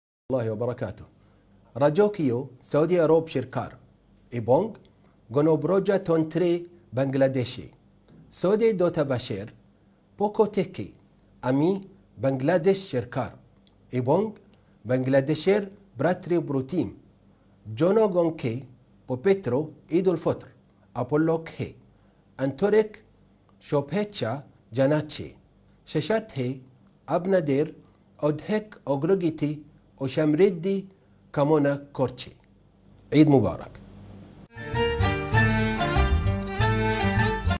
বাংলা ভাষায় বাংলাদেশে নিযুক্ত সৌদি রাষ্ট্রদূত ঈসা বিন ইউসুফ আল দুহাইলানের ঈদ শুভেচ্ছা || EID MUBARAK
বাংলা ভাষায় বাংলাদেশে নিযুক্ত সৌদি রাষ্ট্রদূত ঈসা বিন ইউসুফ আল দুহাইলানের ঈদ শুভেচ্ছা